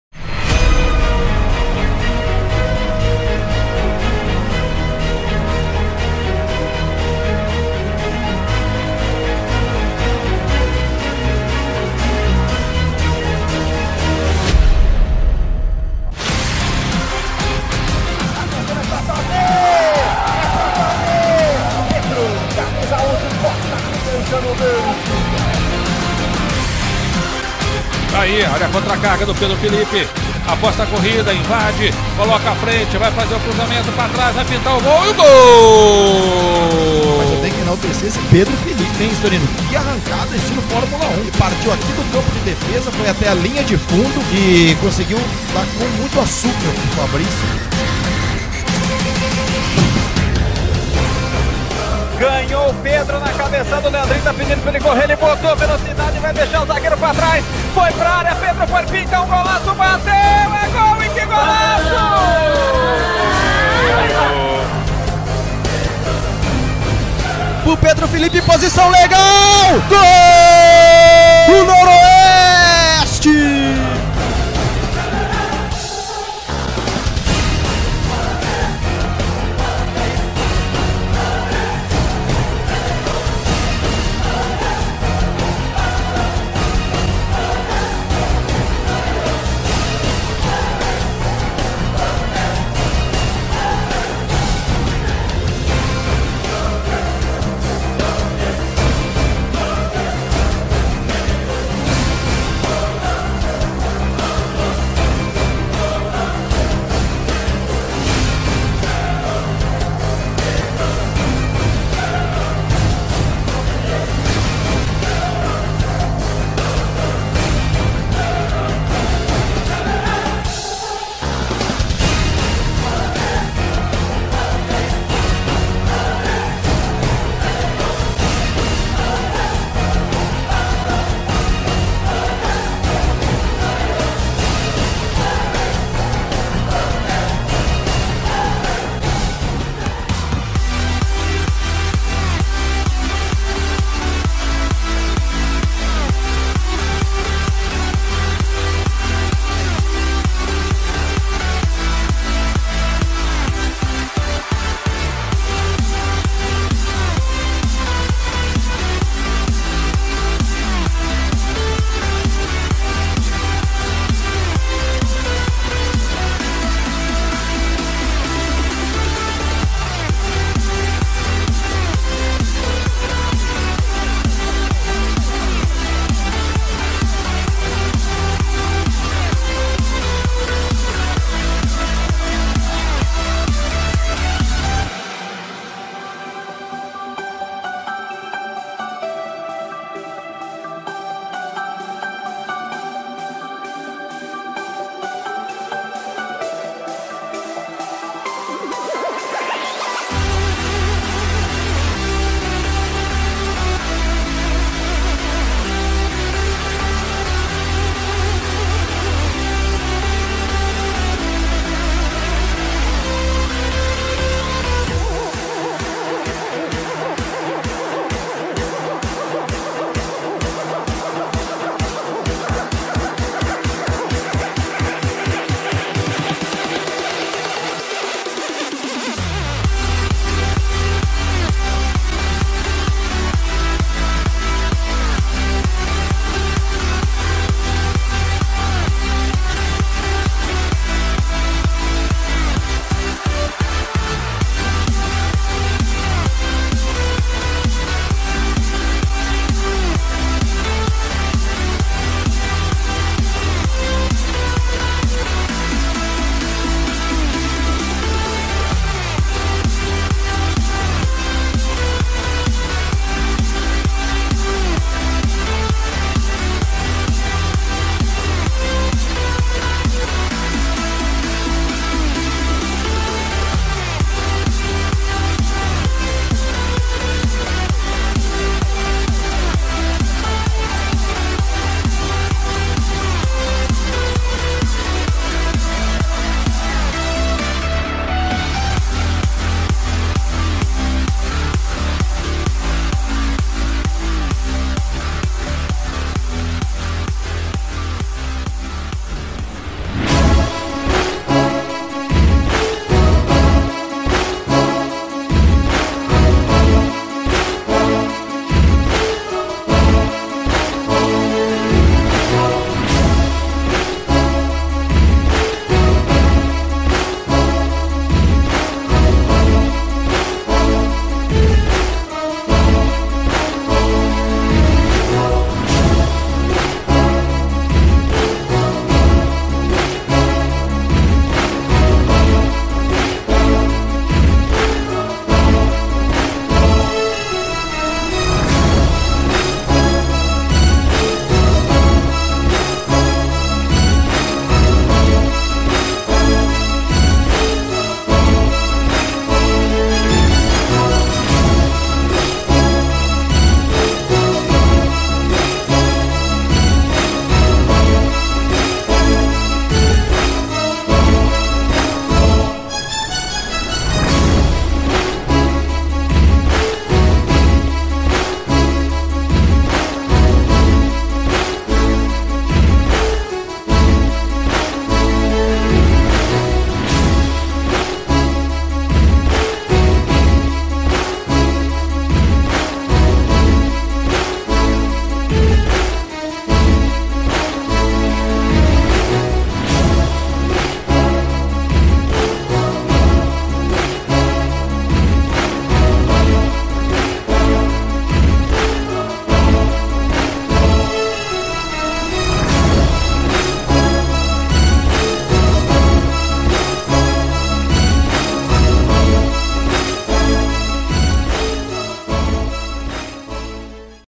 Pedro Felipe - Striker - 2020
Melhores momentos do atleta Pedro Felipe 2020